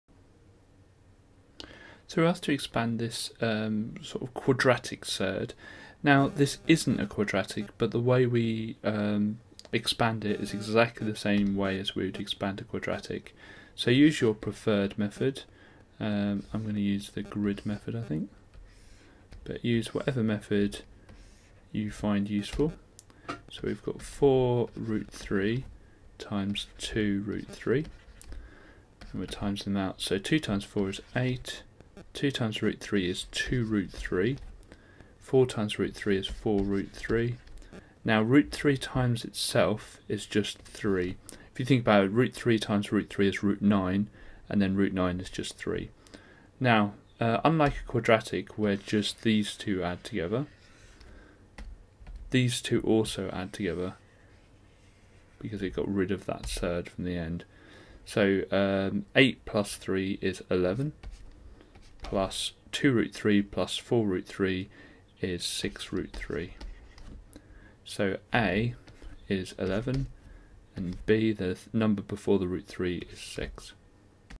Surds: Expand Double Bracket (Both Positive) (Grade 8) - OnMaths GCSE Maths Revision
Topic: Surds: Expand Double Bracket (Both Positive)
Grade: 8
This question appears on non-calculator higher GCSE papers.